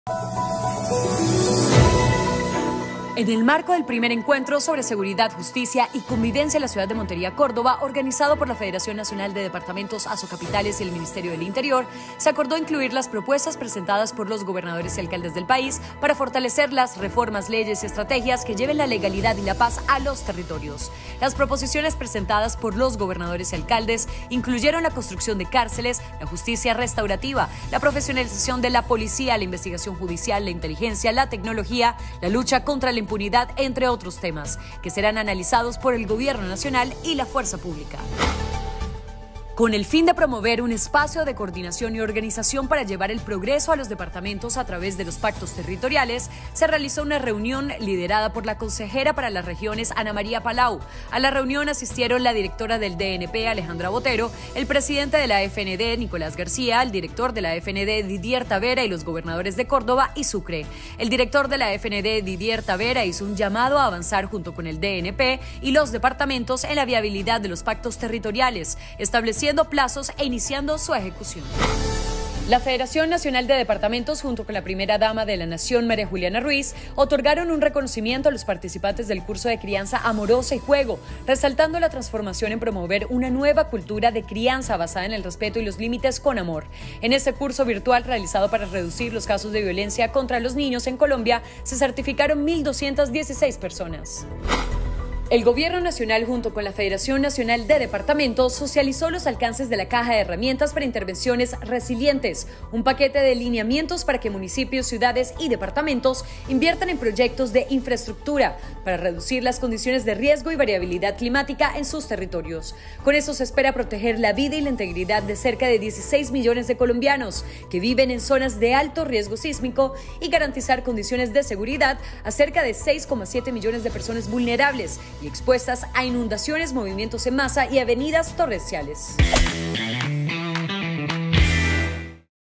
Las BrevesDeLaSemana | 18 al 22 de octubre
🔹️ Se concertan propuestas para fortalecer seguridad, justicia y convivencia en los territorios

🔹️Reunión de Pactos Territoriales

🔹️Reconocimiento de la CrianzaAmorosa +Juego